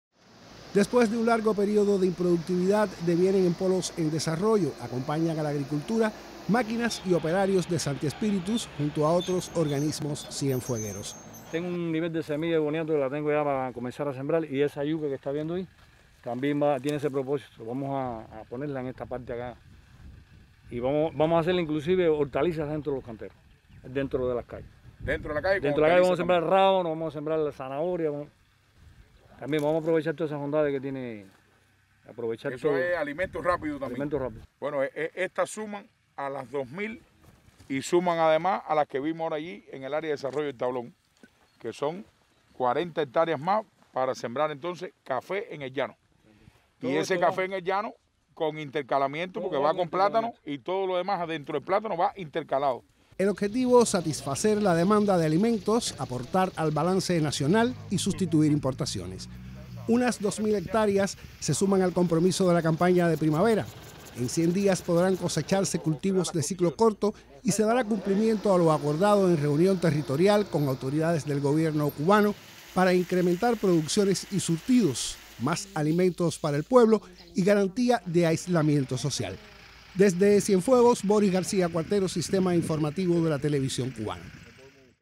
Incrementan áreas agrícolas en Cienfuegos para producir alimentos y garantizar aislamiento social
En Cienfuegos se alistas nuevas áreas agrícolas para producir alimentos y garantizar el aislamiento social. Así lo constataron el Presidente del Consejo de Defensa Provincial, Félix Duartes Ortega y el Gobernador Alexandre Corona Quintero.